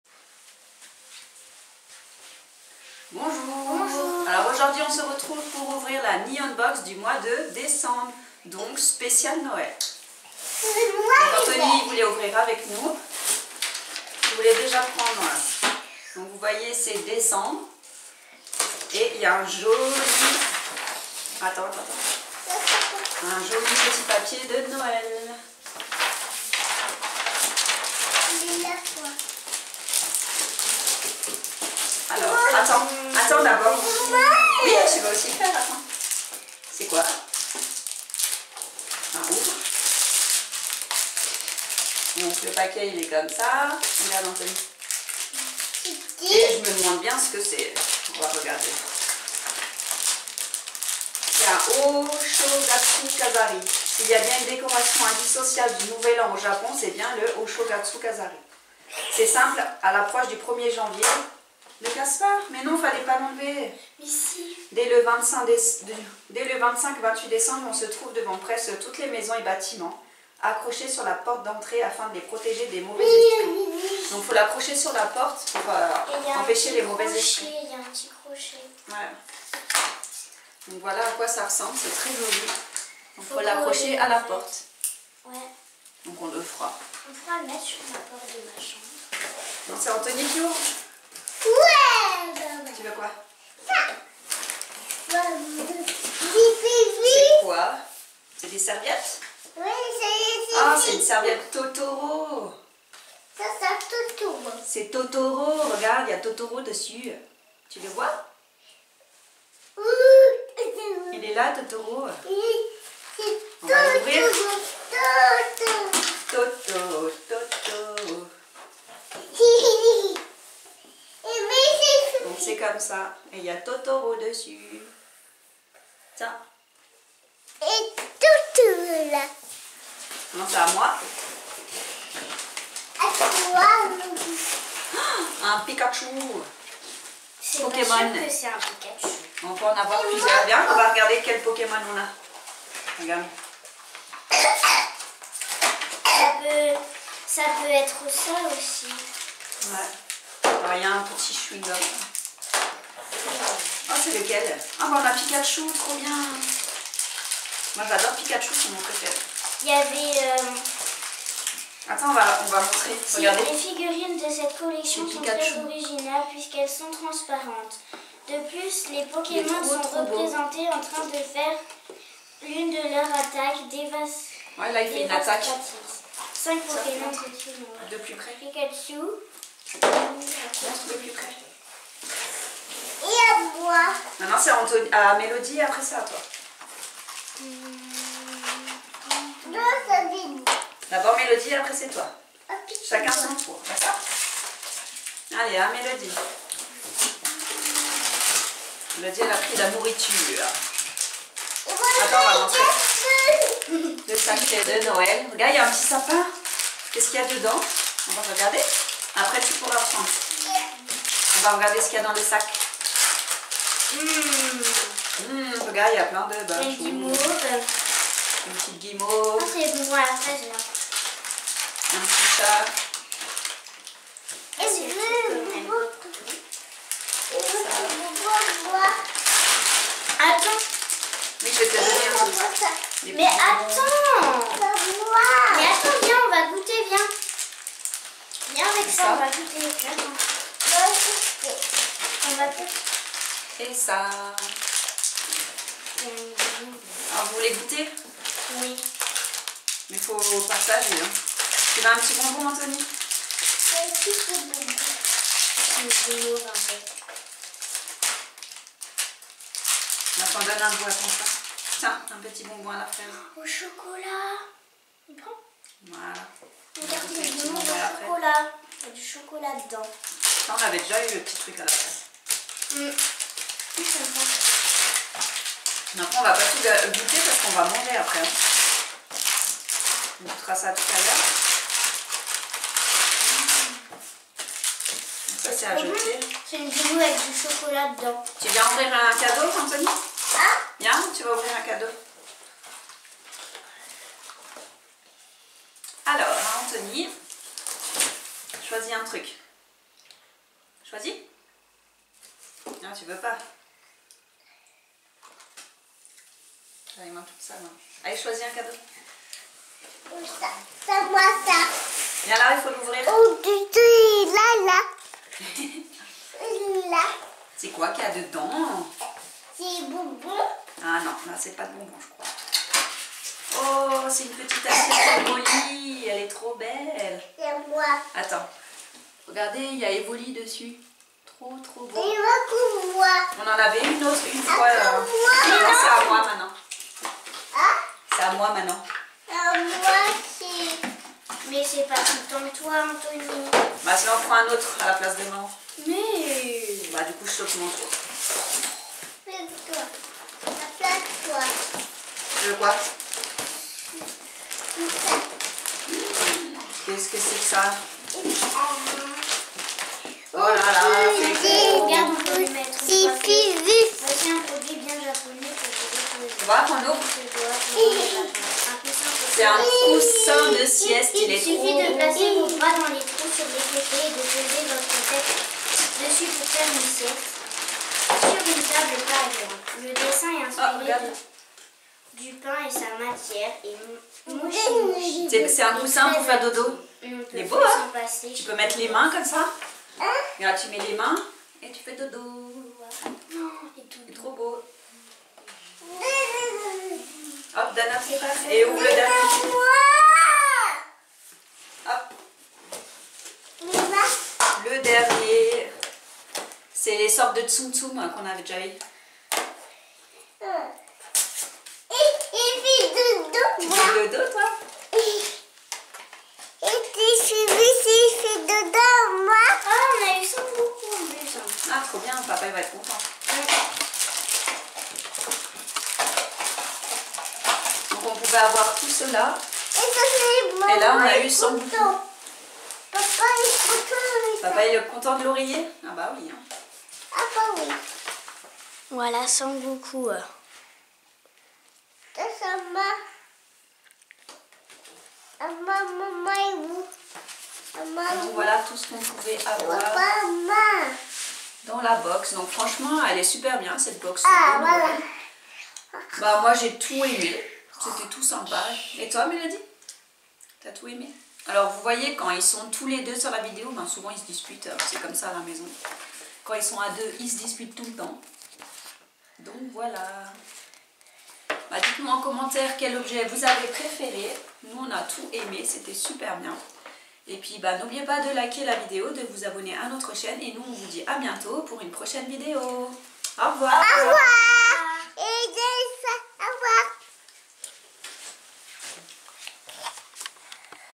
Ouverture de la Nihonbox decembre 2018
Bonjour aujourd'hui on se retrouve pour l'ouverture de la Nihonbox :)